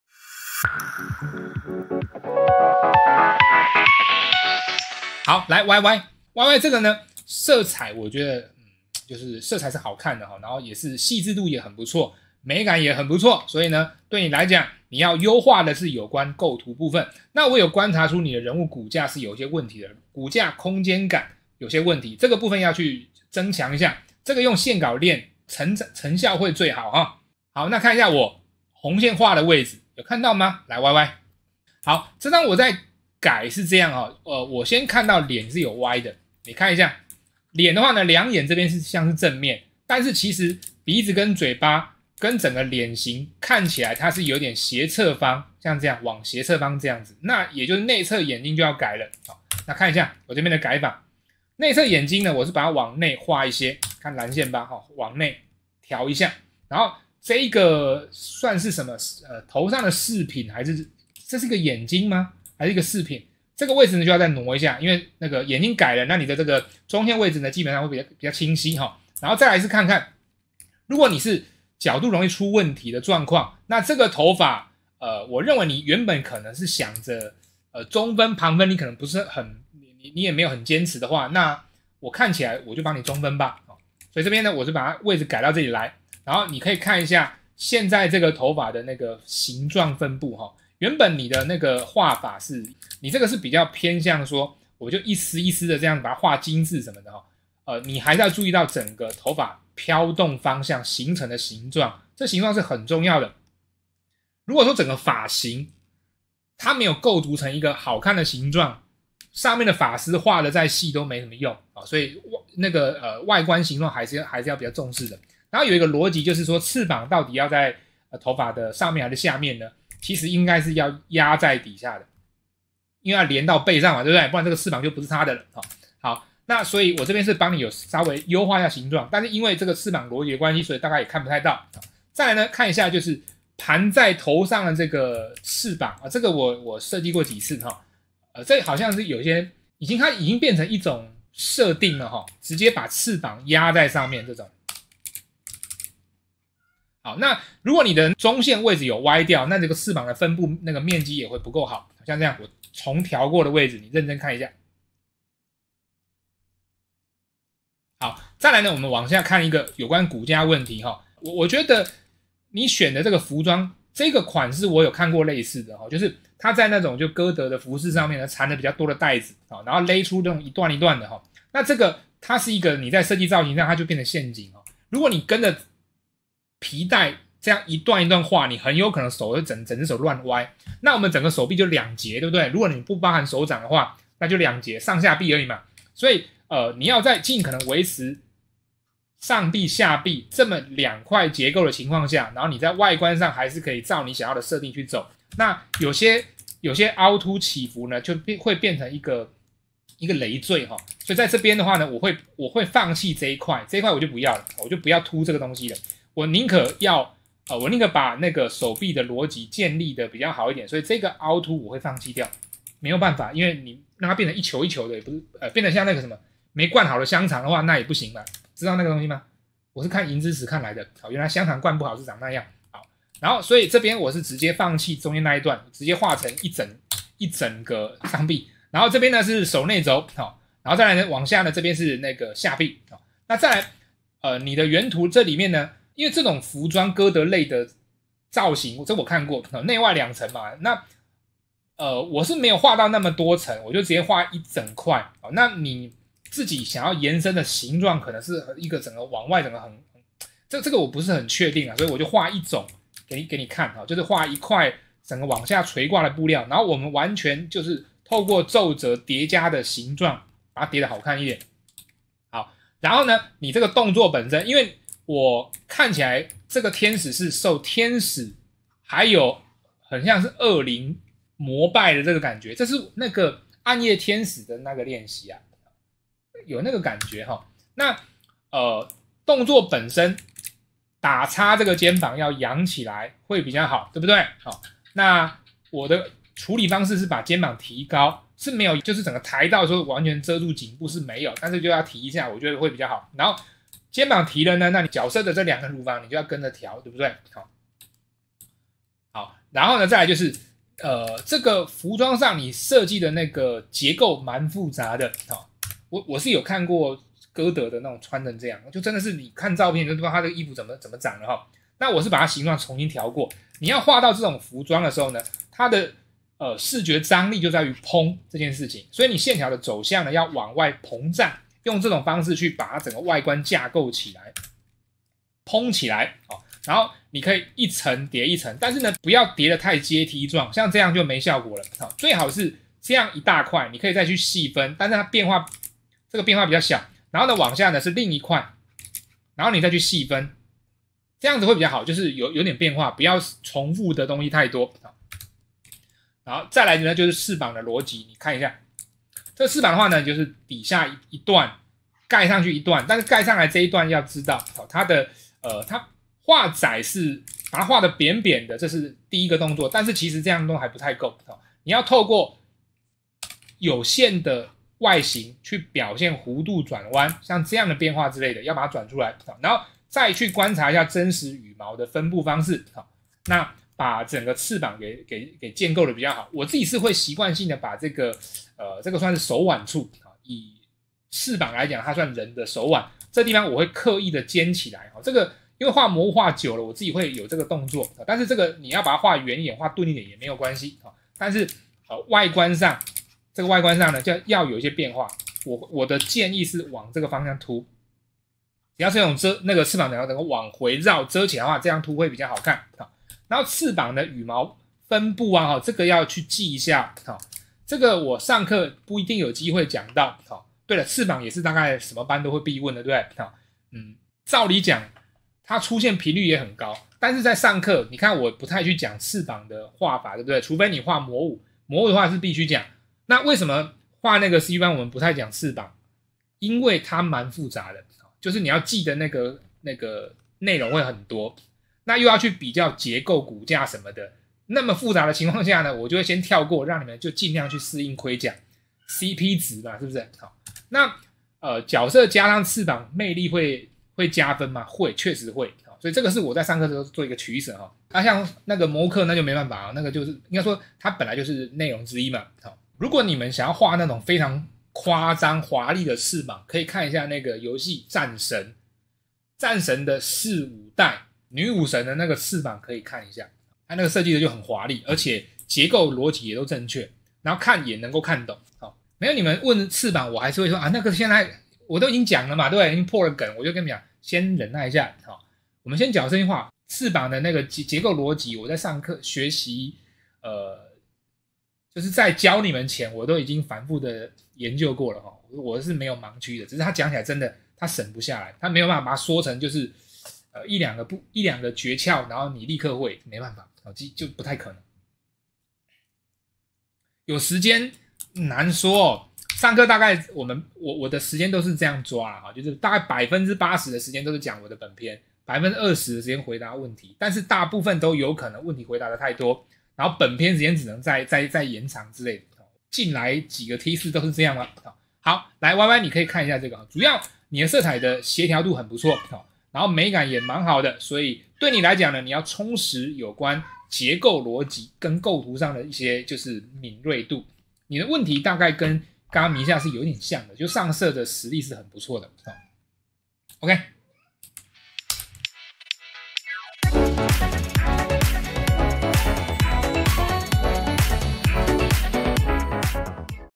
【Twitch改圖投稿】你的翅膀插畫角色還能更好看！歌德系多布料結構要如何畫的平衡？
2024 最新夏季電繪達人培訓課程 限時招生中

每週六晚間8點半
Youtube創作實況 :  @antilousart